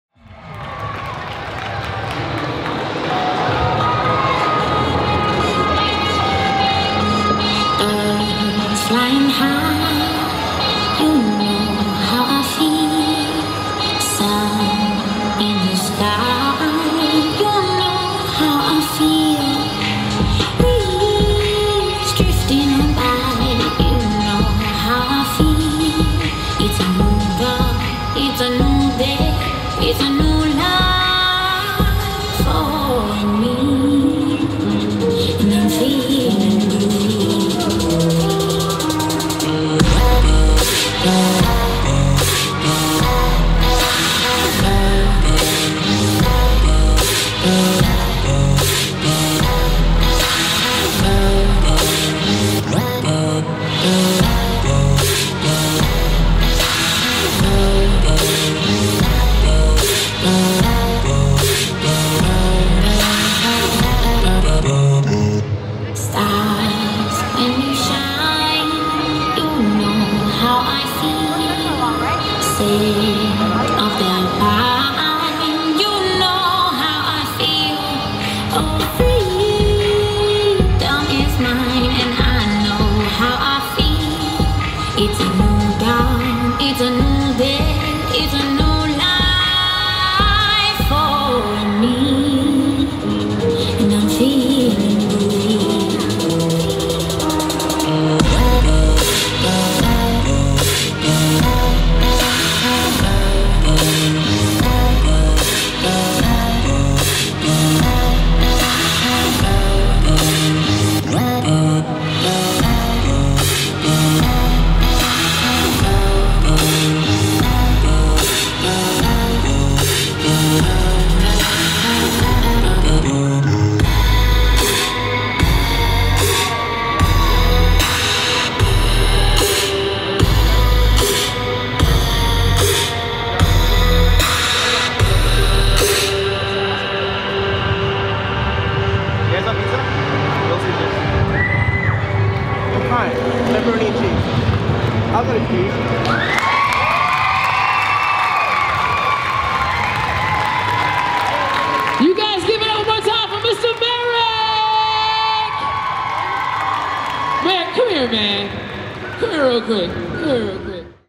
MERRICK | Urban Street Jam 2017 [@VIBRVNCY]
MERRICK | Exhibition
Urban Street Jam 2017 Presented by Stylz Dance Studio & Vlado Footware
Anaheim Convention Center 3.4.17